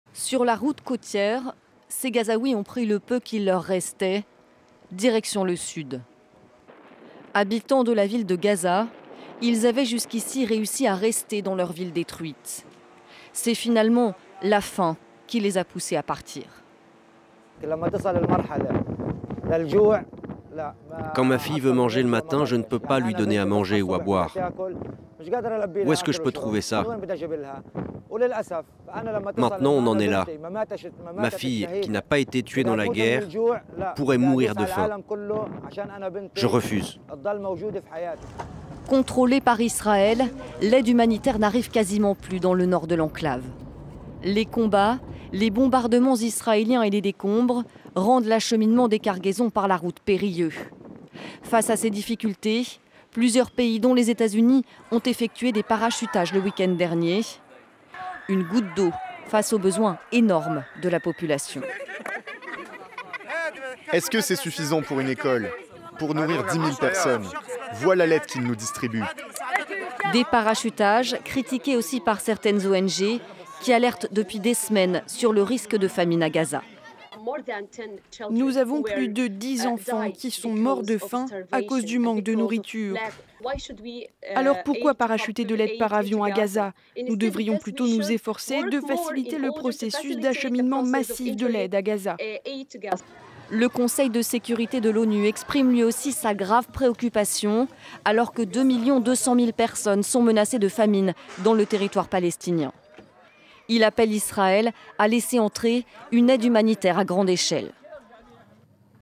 Négociations de trêve à Gaza : 2,2 millions d'habitants menacés de famine • FRANCE 24
Les négociations sur une trêve dans la guerre entre Israël et le Hamas palestinien reprennent lundi au Caire après un "progrès significatif", au lendemain d'un appel de la vice-présidente américaine Kamala Harris à un "cessez-le-feu immédiat". Selon l'ONU, 2,2 millions des 2,4 millions d'habitants sont menacés d'une famine "quasiment inévitable", d'après Jens Laerke, porte-parole de l'Ocha, l'agence de coordination des affaires humanitaires des Nations unies.